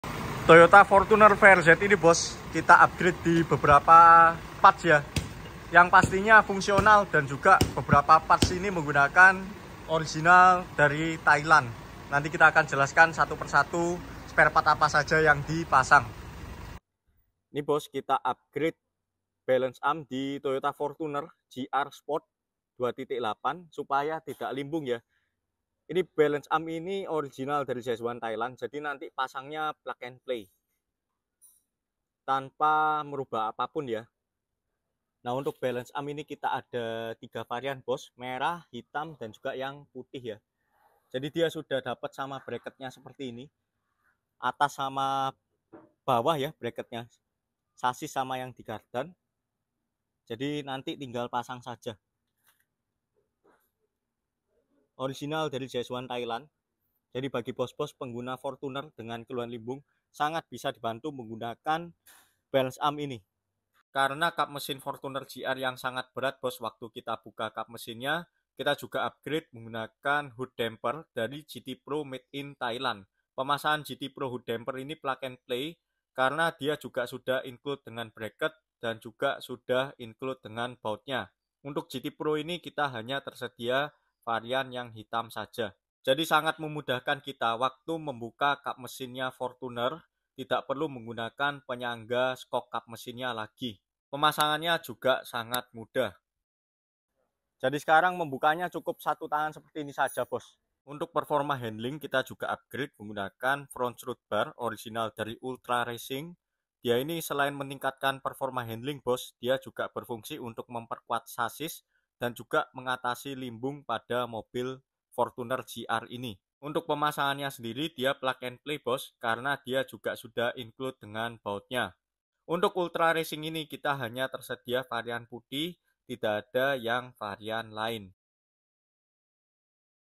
Modifikasi Fortuner GR Anti Limbung | Budispeed
Modifikasi Yang di Pasang :

-.Balance Arm
-.Front Strut Bar
-.Hood Damper

Budispeed
Manyar Kertaadi 6/6, Surabaya